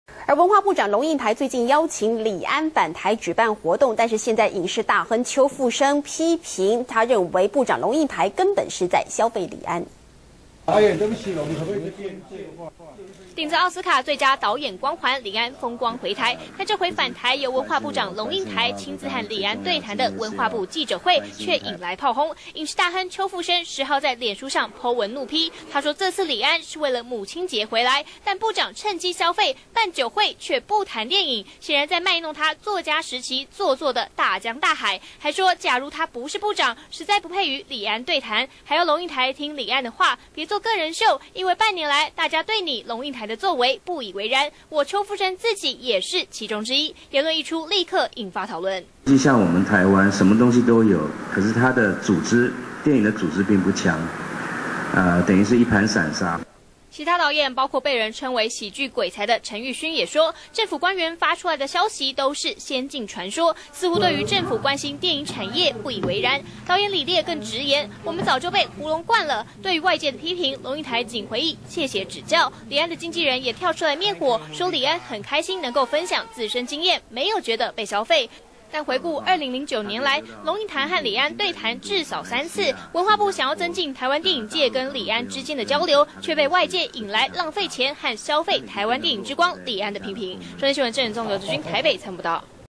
中天新聞》邱復生臉書怒轟龍應台 別做個人秀
中天新聞》文化部長龍應台邀李安返台，舉辦活動，被外界批評，影視大亨邱復生也在臉書怒批部長龍應台，根本就是在消費李安，甚至說如果龍應台不是部長，實在不配與李安對談，對此龍應台僅短短回應謝謝指教，會再做給大家看。